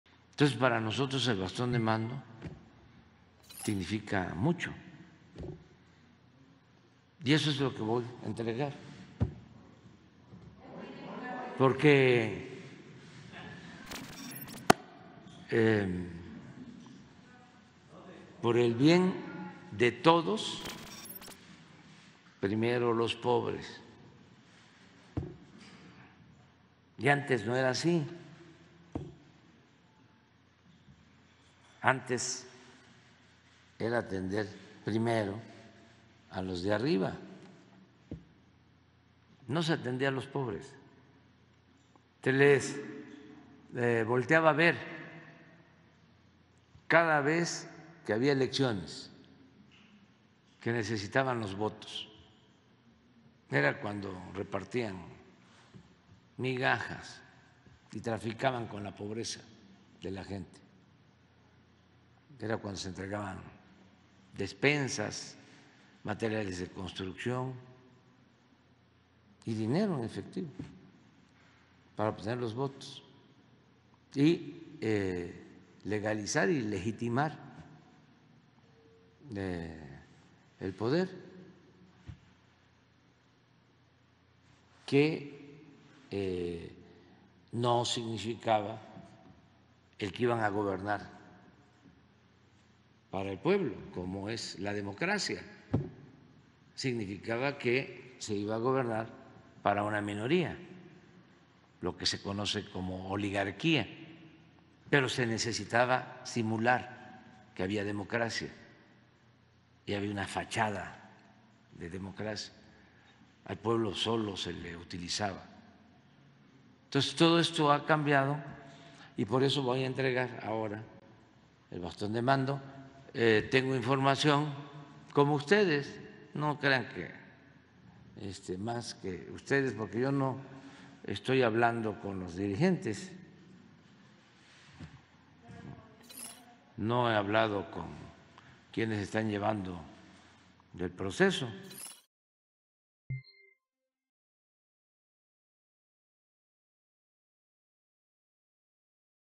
AMLO mantiene incógnita sobre entrega de bastón de mando a próximo coordinador de la 4T
El presidente Andrés Manuel López Obrador manifestó que el bastón que entregará al ganador o ganadora de la encuesta de Morena para ser coordinador de la 4T “es un símbolo de lucha por los más pobres”